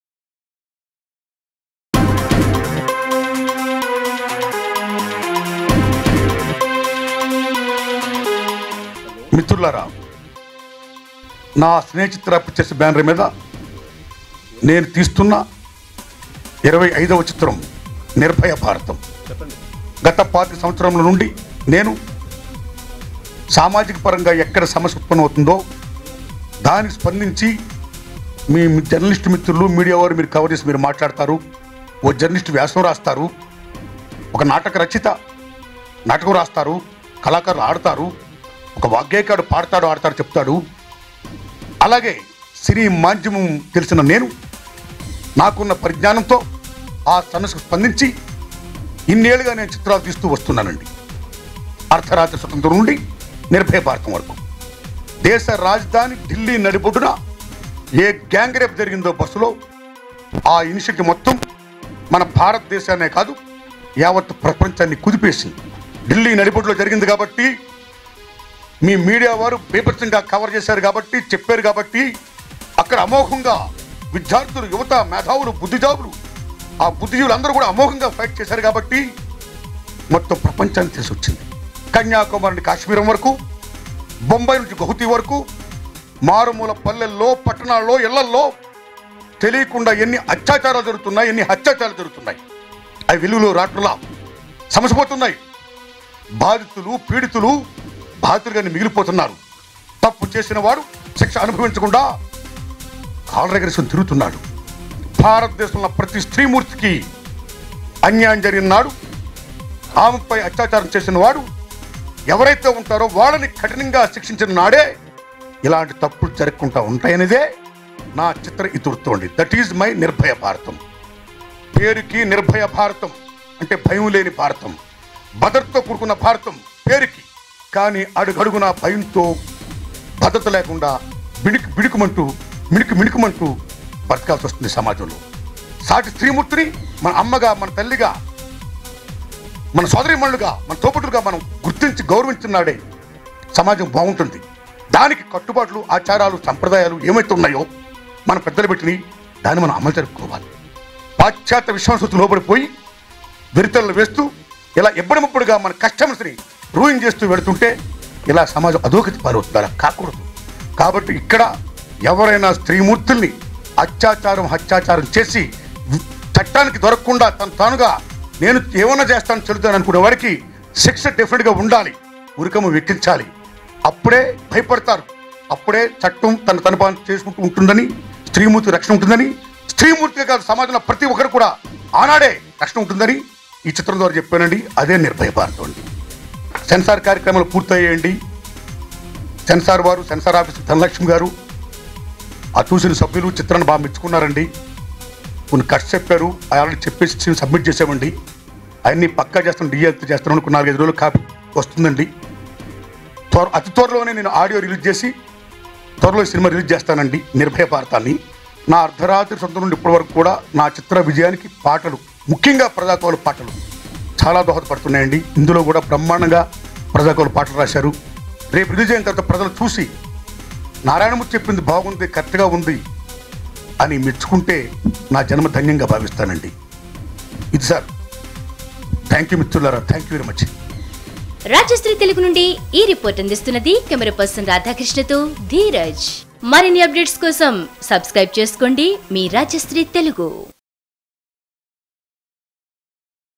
Narayana Murthy About His New Movie Nirbhaya Bharatham [HD]
RajshriTelugu Narayana Murthy About His New Movie Nirbhaya Bharatham [HD]. Check out what Narayana Murthy has to say about his upcoming movie Nirbhaya Bharatham, in this HD 1080p video.